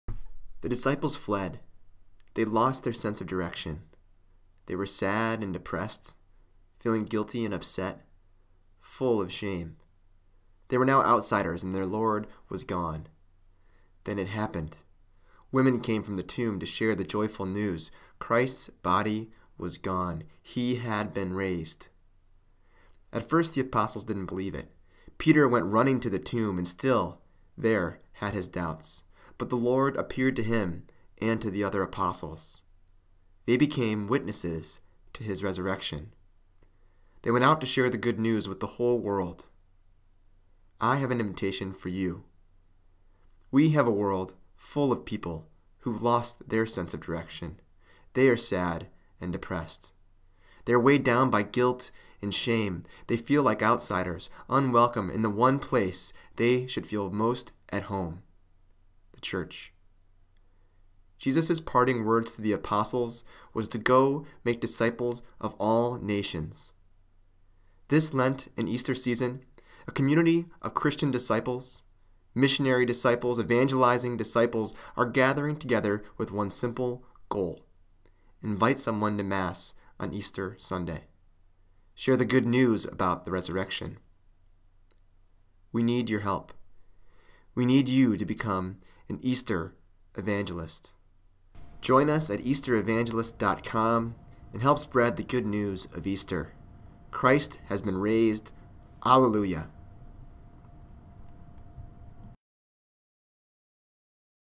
Easter Evangelist
Your invitation might be the one thing that can transform someone's life.

Our Goal: Extend 5,000 personal invitations to Easter masses.

Easter Evangelists commit to:

1. Invite someone to Mass on Easter Sunday (or Saturday)
2. Share why Easter is important to them.

The purpose of the Easter Evangelist campaign is to gather a dedicated community of disciples throughout the world in a common goal to share the good news of the Resurrection.